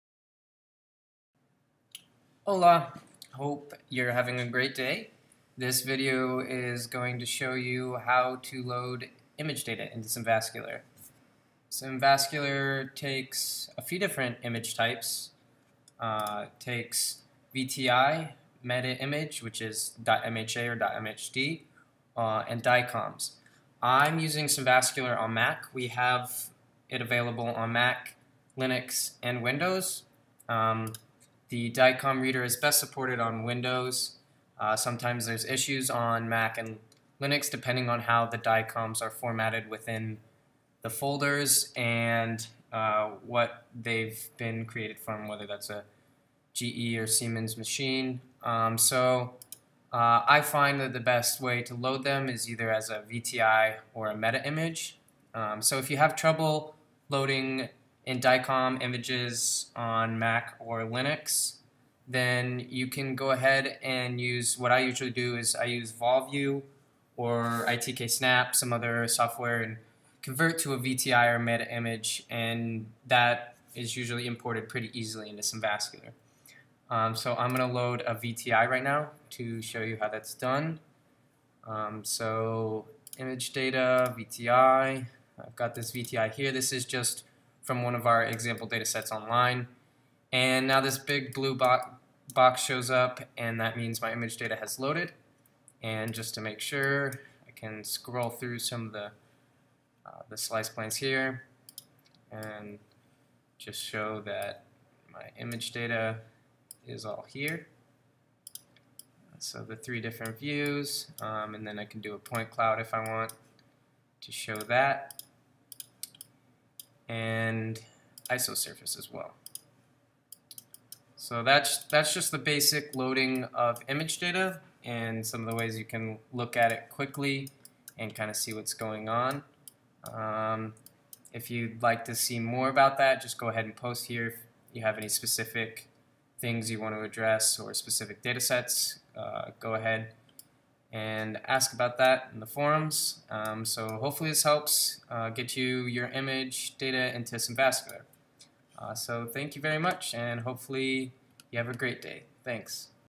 Loading Image Data
Image file types accepted and how to load them